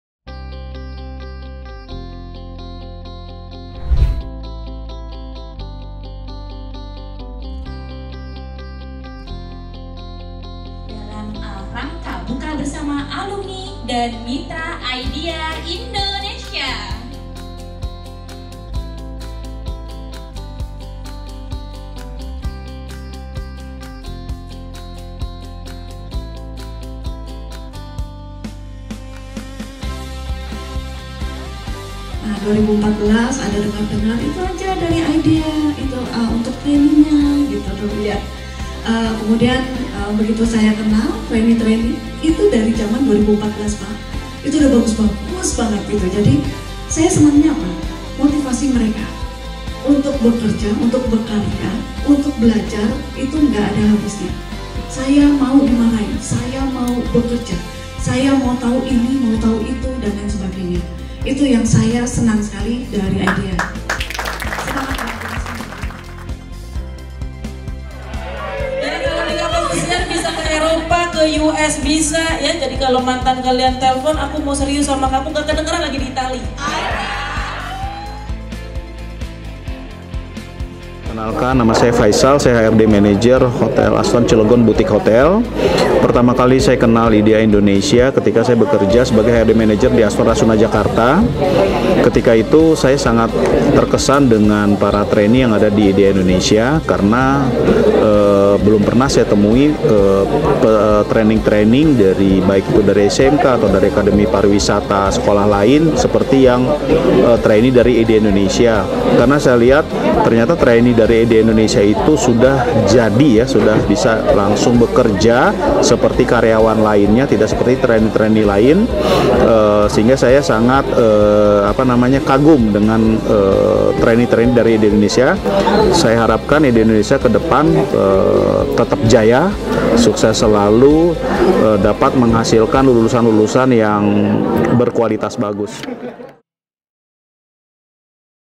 JOB FAIR IDEA INDONESIA 2023 - Diikuti 20 Mitra Industry dengan 50 Posisi Tersedia
PT IDeA Indonesia Tbk kembali menggelar Jobfair Tahun 2023 di Hotel Jambuluwuk Thamrin Jakarta Pusat, Selasa 28 Maret 2023.

Kegiatan ini diikuti oleh 20 Mitra Industry dengan membuka kurang lebih 50 Posisi yang tersedia dan Para Alumni-alumni Idea sangat antusias mengikuti acara Jobfair ini.

IDEA: The World Class Hospitality Company